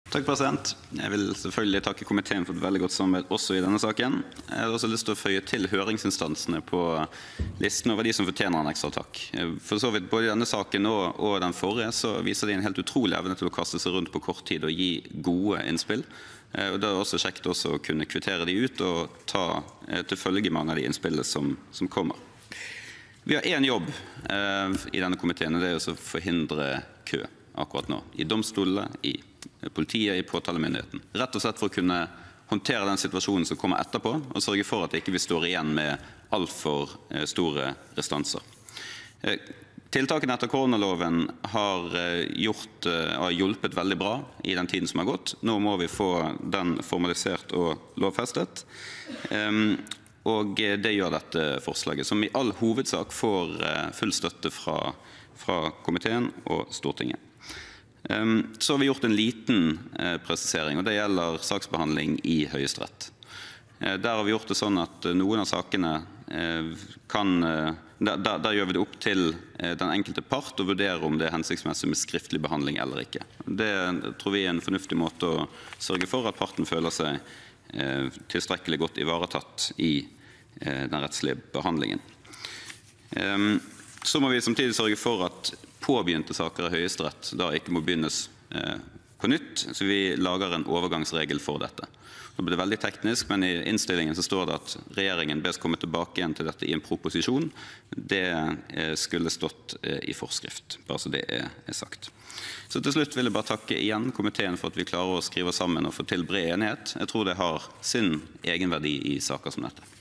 19.05.2020 Sak 6 1 Peter Frølich
Om Midlertidig lov om tilpasninger i prosessregelverket som følge av utbruddet av covid-19 mv. (Innst. 275 L (2019–2020), jf. Prop. 94 L (2019–2020))